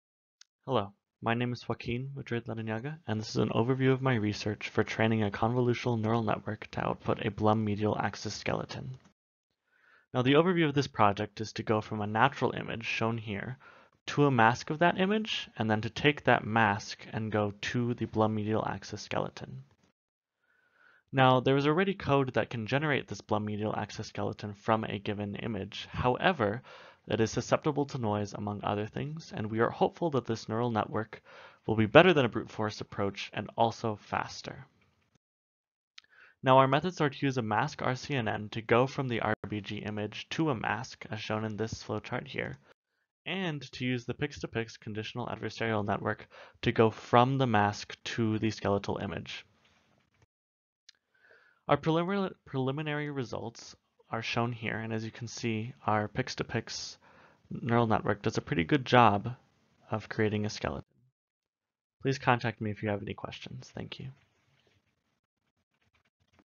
Training A Convolutional Neural Network to Output a Blum MedialAxis Skeleton - An Overview
This is a one minute overview of my research regarding "Training A Convolutional Neural Network to Output a Blum MedialAxis Skeleton" in collaboration with Say Brown under the direction of Kathryn Leonard at Occidental College.  This is in preparation for the 2020 SACNAS STEM conference.